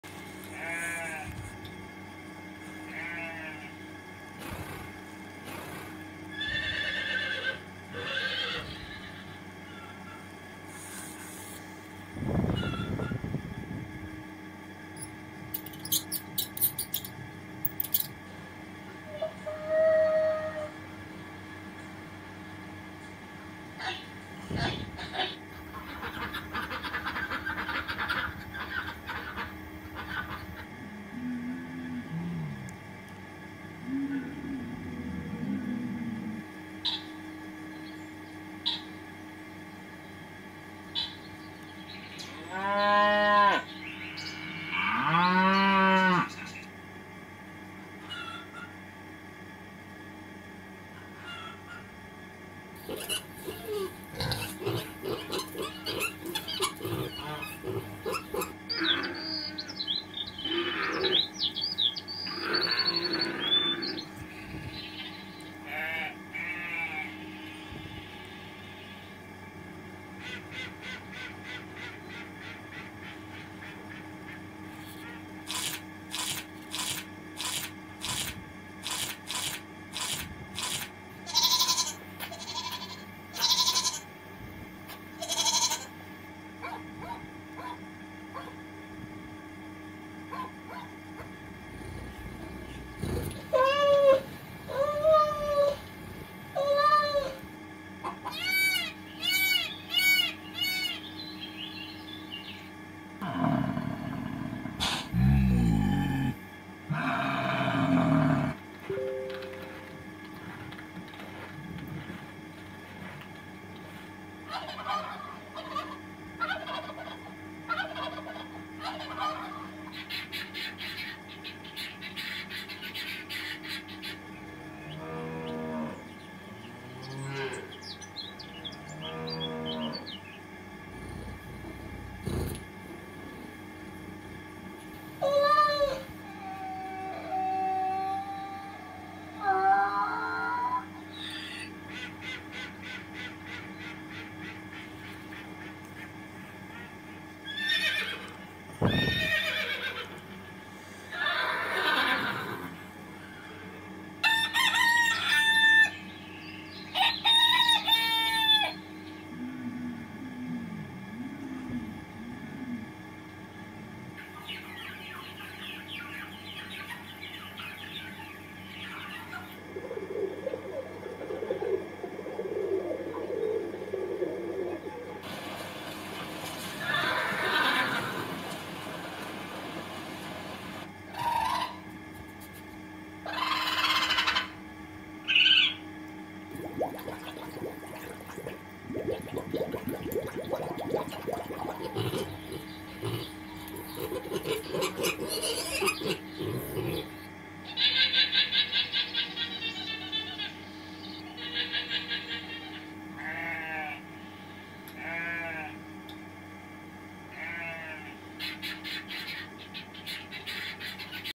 Colorful sounds | animals
Cute moments of animals as babies: monkey, duck, cow, chicken, cat, elephant, etc...

This is what motivates us to make improvements to the channel. Thanks for listening & watching